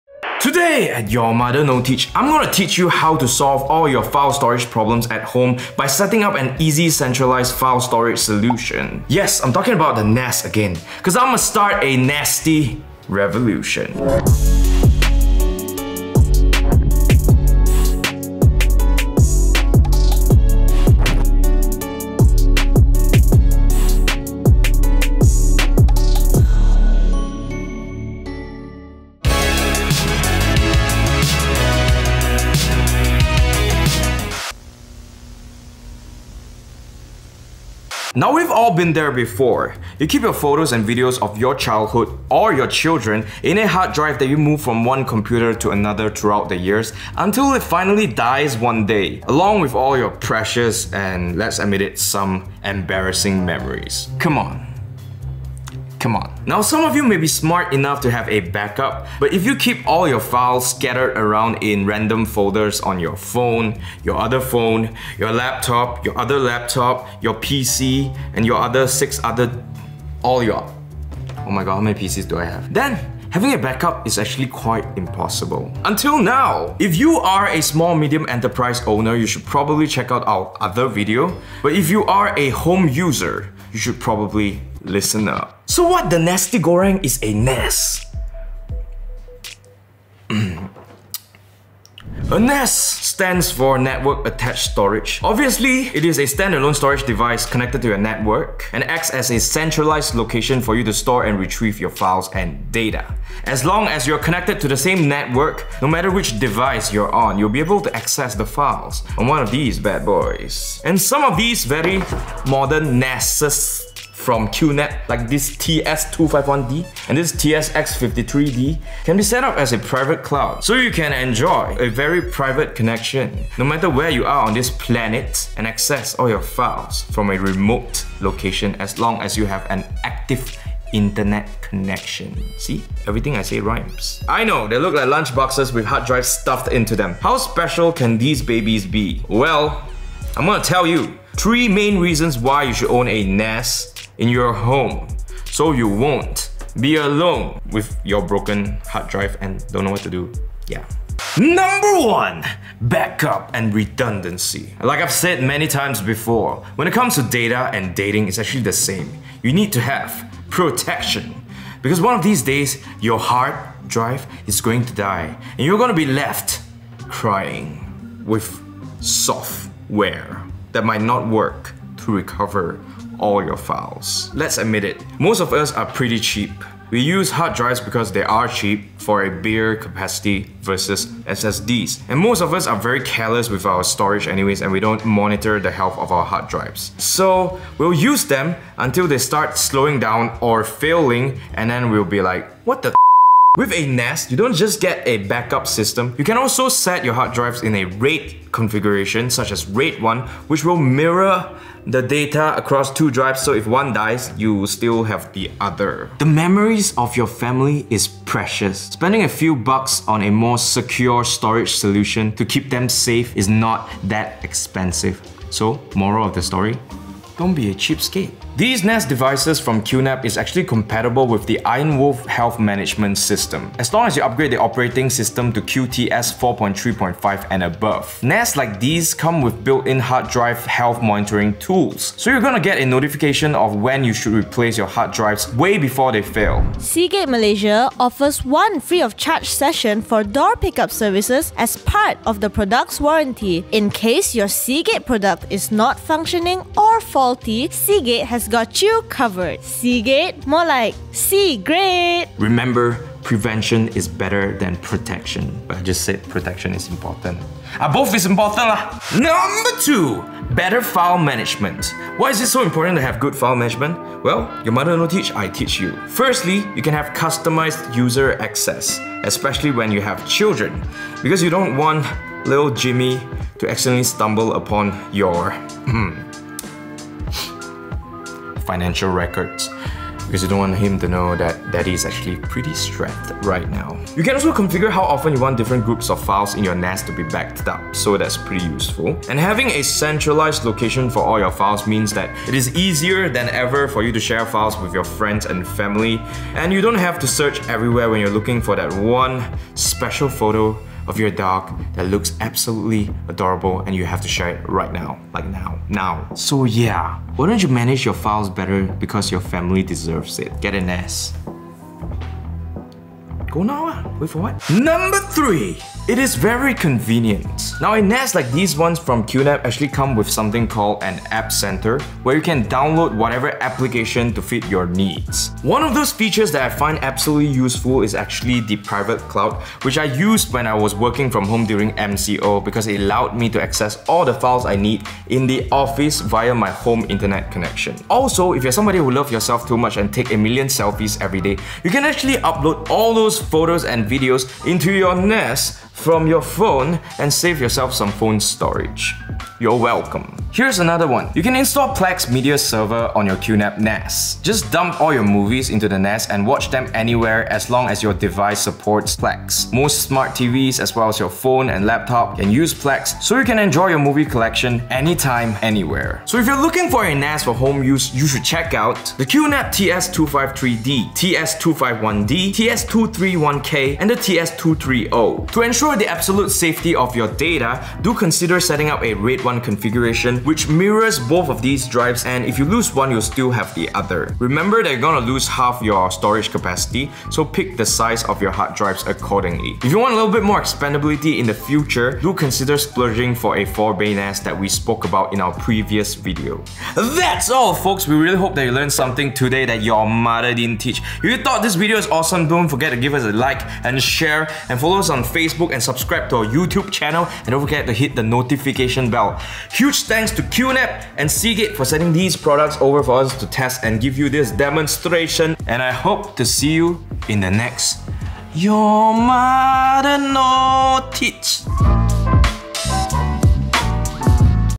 Should you get a Network Attached Storage (NAS)?
Did your mother teach you what a home server is? Your mother no teach, I teach!

Today at Your Mother No Teach, I will teach you how to solve all your file storage problems at home with 1 easy centralized storage solution.

Yes… we're talking about the NAS again 'cause I'mma start a NAS-ty revolution.

Here are the 3 main reasons why you should use a NAS at home!
#1 Backup and Redundancy
#2 Better File Management
#3 Convenient Features

If you're looking for a NAS for home use, we recommend at least a 2-bay NAS like this QNAP TS-253D or TS-251D. To ensure that your data is safe, use a RAID 1 setup that mirrors both of the drives so if one drive fails, you still have the other one but you do lose half your storage capacity. If you have a bit more to spend, you can also go for a 4-bay option which gives you more expandability in the future.

Also, don’t forget to use hard drives that are designed for NAS purposes such as the Seagate Ironwolf. They are designed to deal with the load of running weeks on end and is capable of running at a fast 7,200rpm spin rate without risk of failure. They are also very quiet with minimal vibration noise without compromising performance. For Home Users, we recommend that you either go for 2x 4TBs or if you have multiple users, splurge for 2x 8TBs!